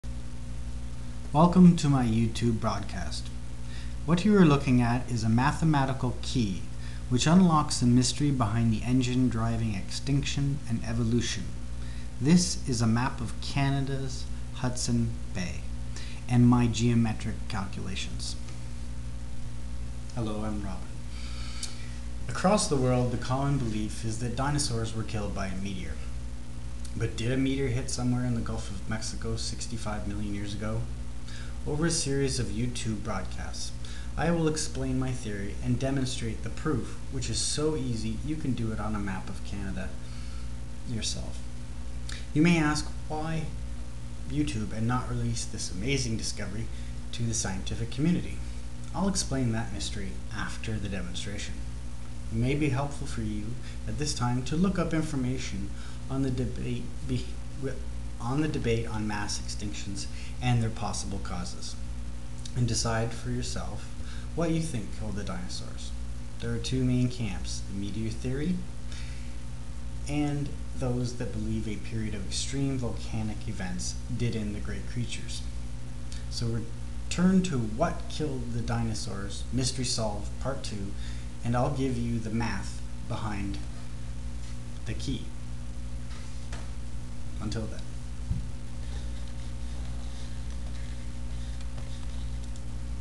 WHAT KILLED THE DINOSAURS? MYSTERY SOLVED PART 1
IN MY YOU TUBE BROADCAST I WILL EXPLAIN MY THEORY OF EXTINCTION AND EVOLUTION AND DETAIL THE MATHEMATICAL PROOF.  THIS SCIENTIFIC DISCOVERY HAS VERY SERIOUS IMPLICATIONS CONSERNING CLIMATE CHANGE, GLOBAL WARMING AND ENVIROMENTAL DEGRADATION. WHATEVER KILLED THE DINOSARS MAY DESTROY CIVILIZATION AND HUMANITY MAY BECOME EXTINCT.  ACT NOW THE FUTURE IS HERE.